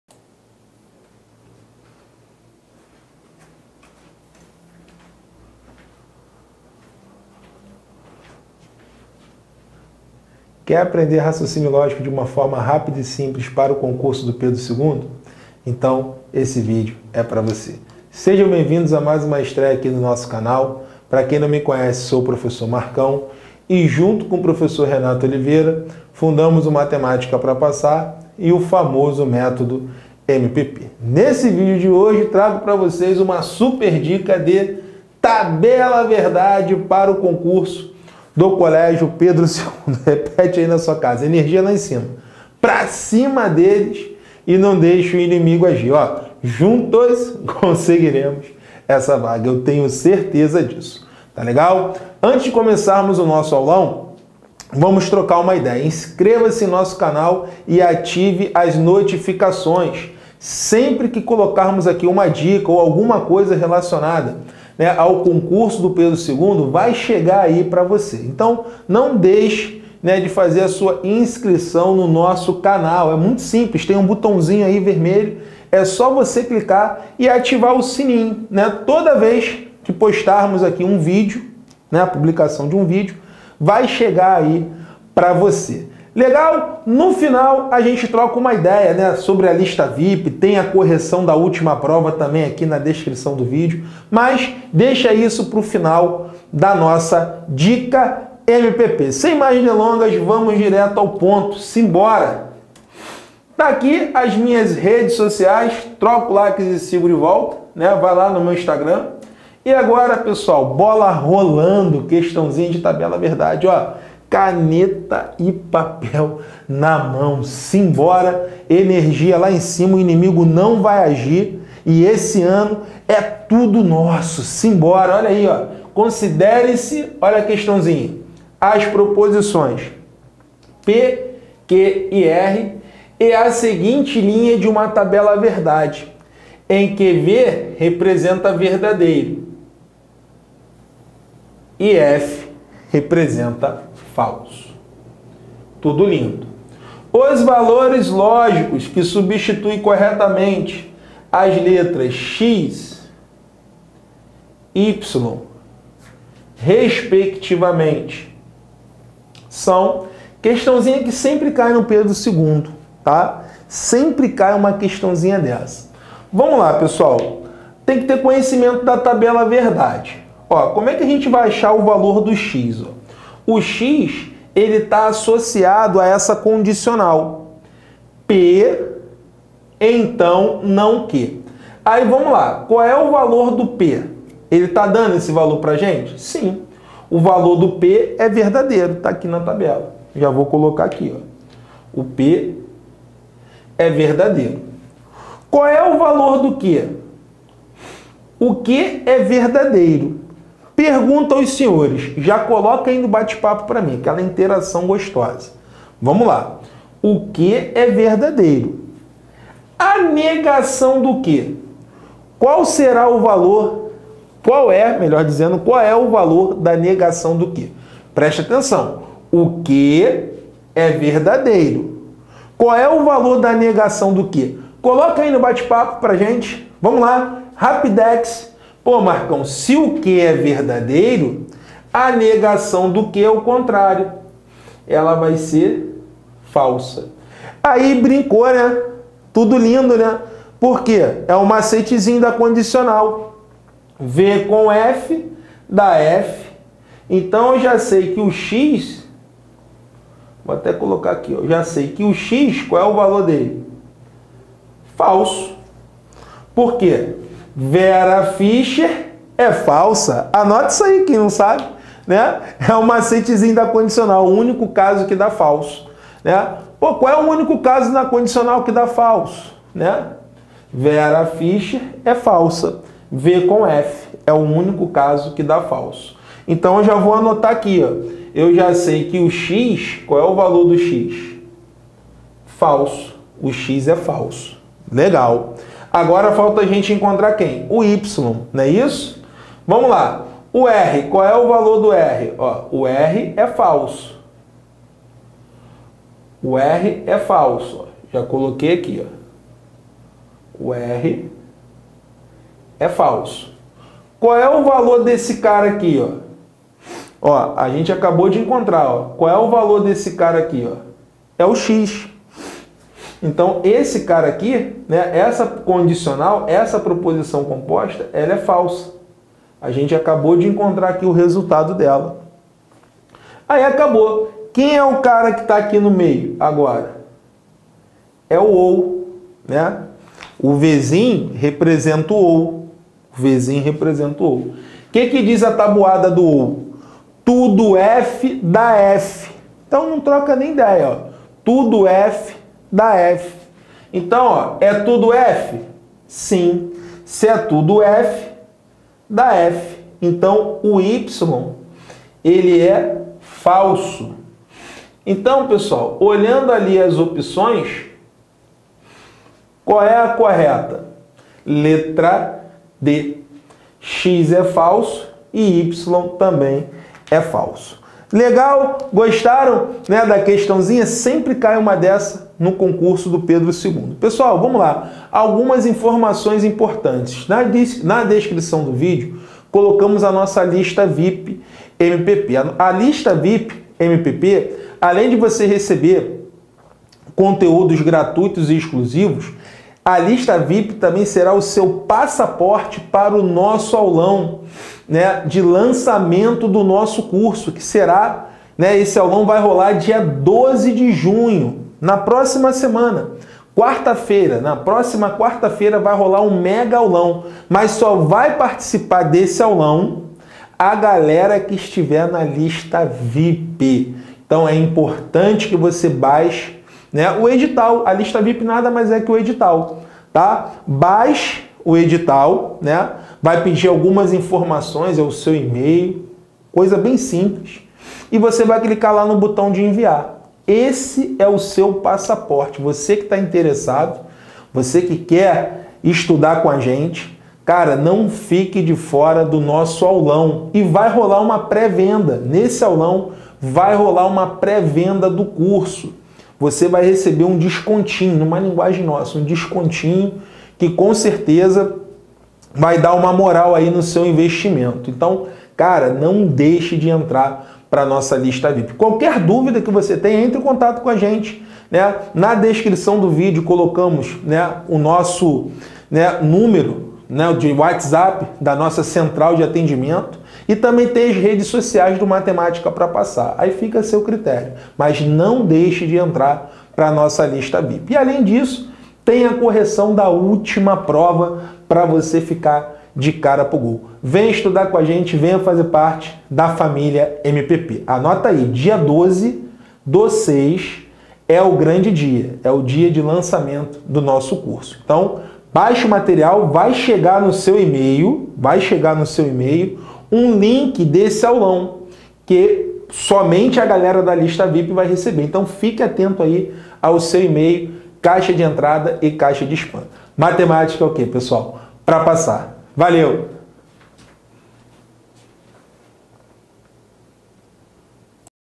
CONCURSO COLÉGIO PEDRO II: APRENDA TABELA VERDADE.[2019]
Nessa super dica nosso professor, MARCÃO, mostra o que você deve fazer para aprender tabela verdade nessa prova e transformar a história da sua vida.